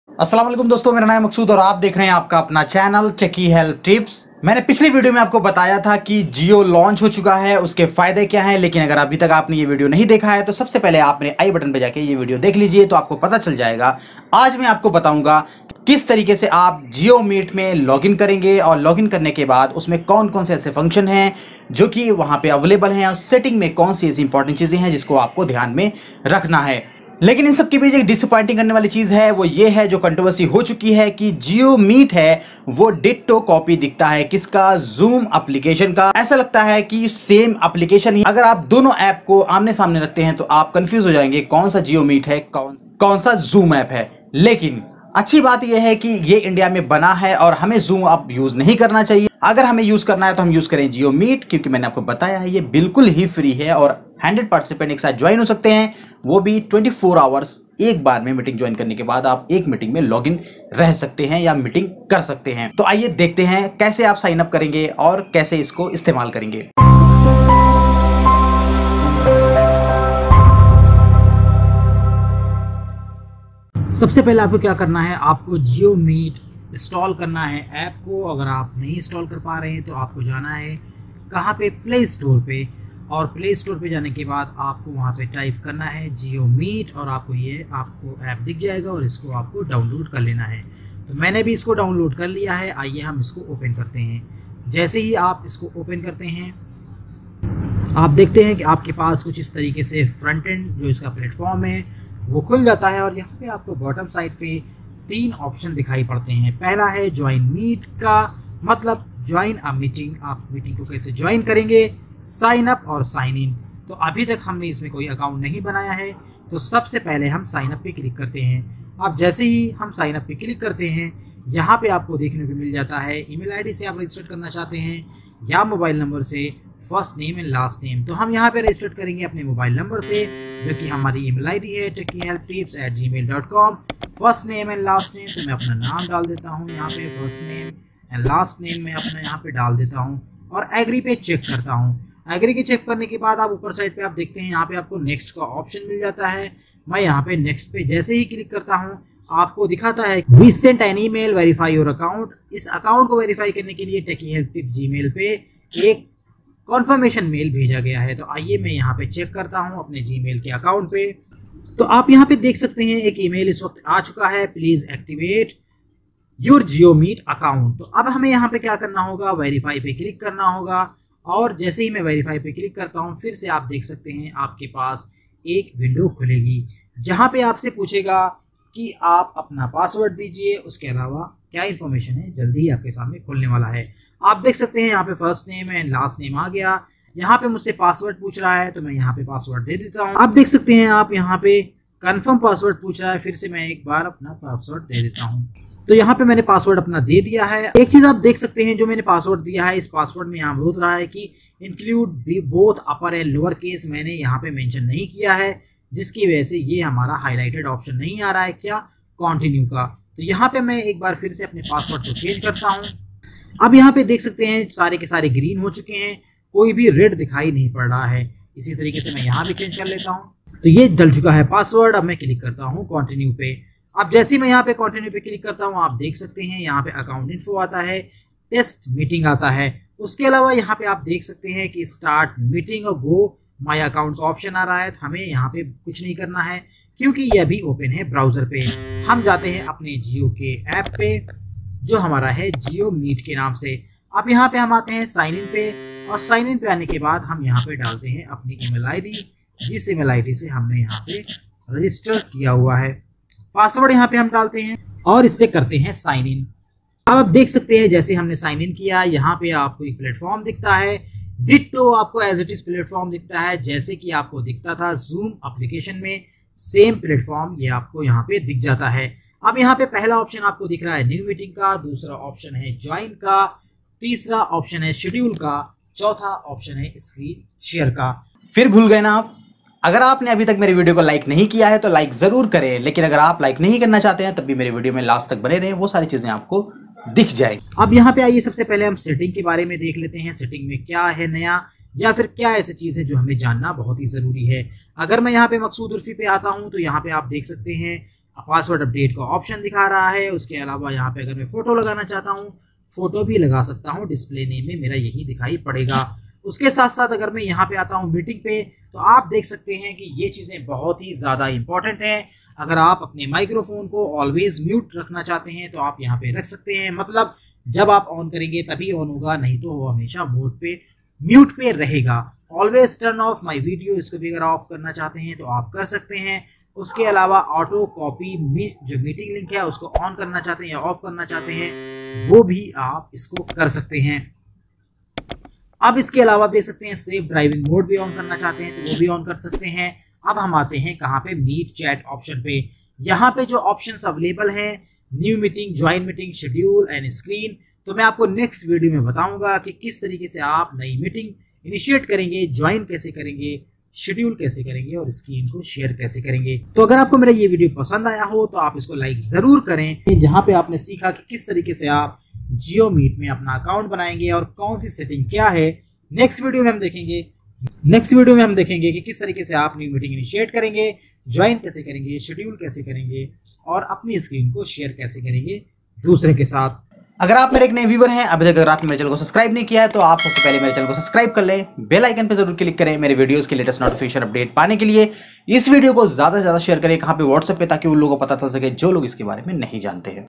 How to Download & Sign up Jio Meet App|How to Create Account In Jio Meet AppJioMeet Conferencing App
Hello guys in this video iam showing how to Download & Sign up in Jio meet App .So it is very easy please watch full video and don't forget to Subscribe my Channel For these types of videos

CAMERA TRIPOD
TRIPOD FOR MOBILE
Mini Gorilla TriPod
Ravish Kumar